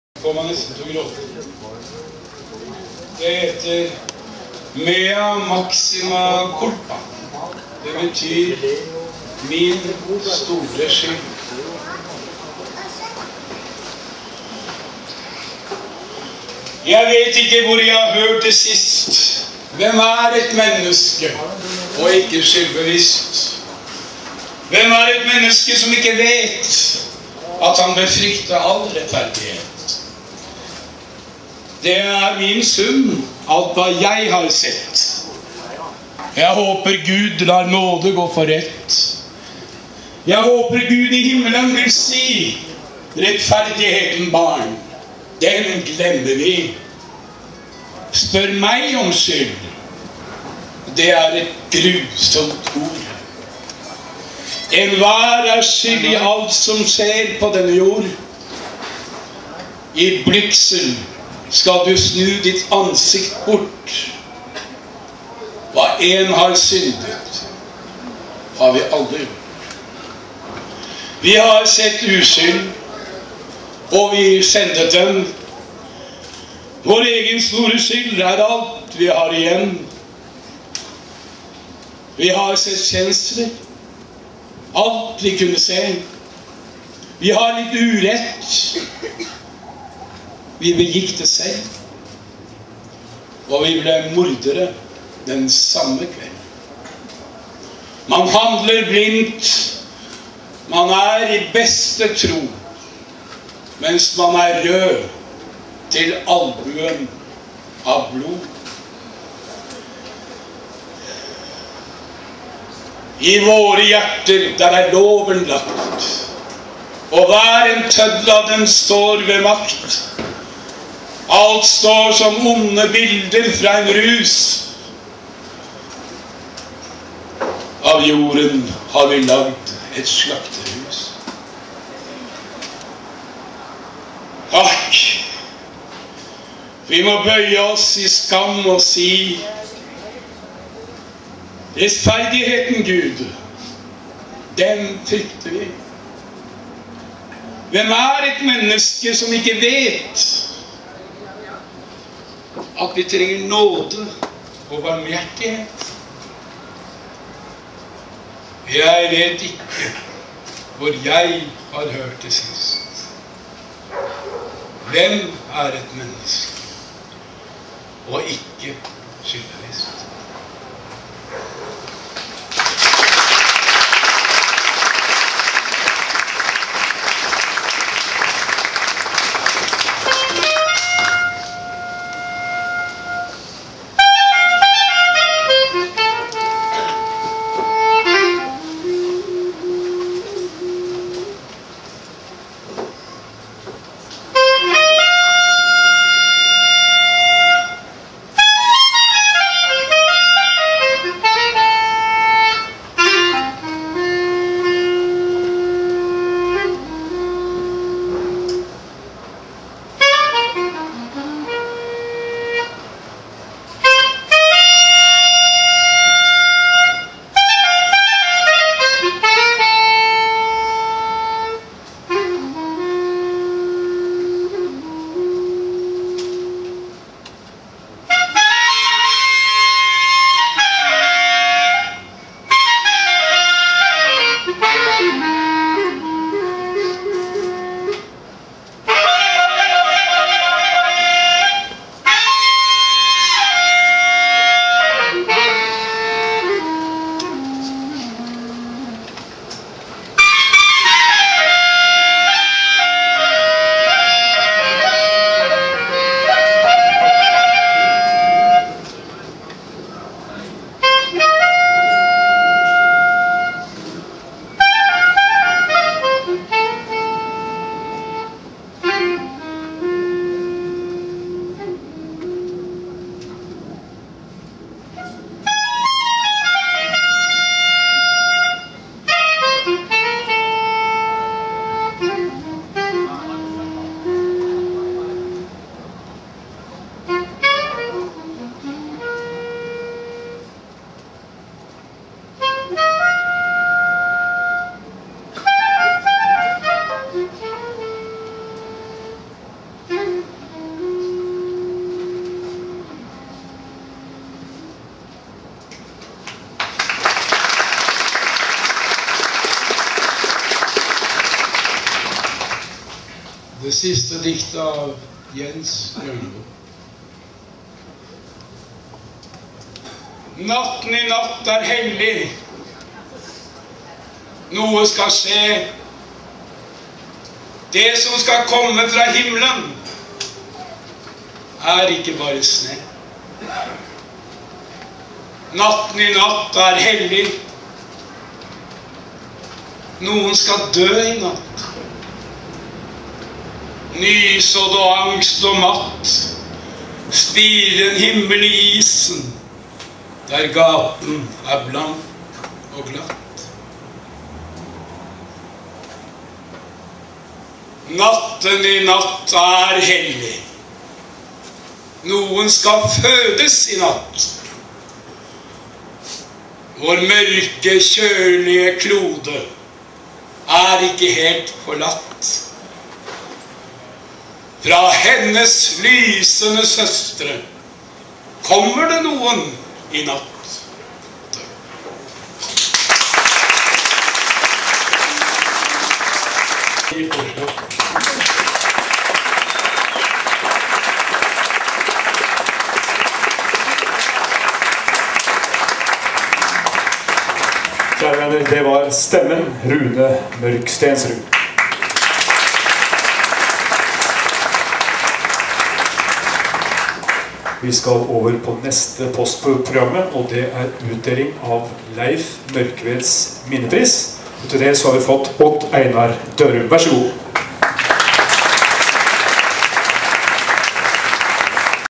Fattigdomshøringen 2009 - Rune Mørk Stensrud leser Bjørneboe + Carl Petter Opsahl, klarinett
Amatøropptak fra Fattigdomshøringen 2009 - Kultur - Rune Mørk Stensrud leser Bjørneboe + Carl Petter Opsahl, klarinett